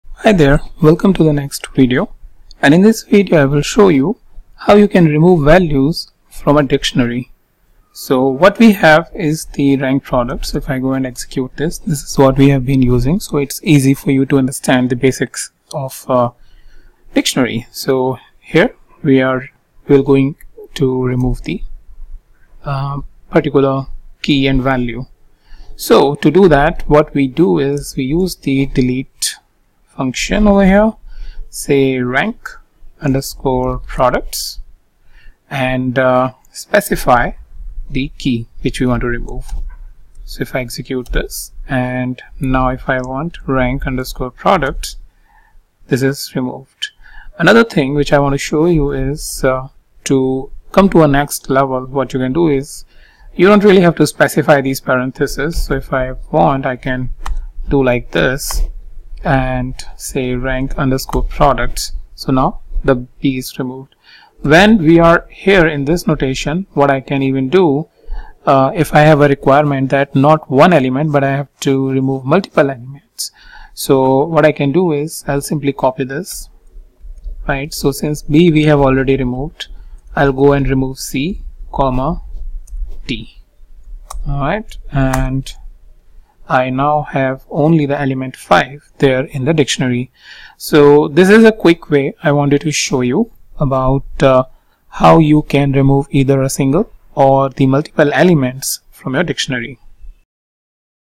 Python Dictionaries Tutorial 4 | How to remove single or multiple elements from dictionary
Python Tutorial 4 - Dictionaries | How to remove single or multiple elements from dictionary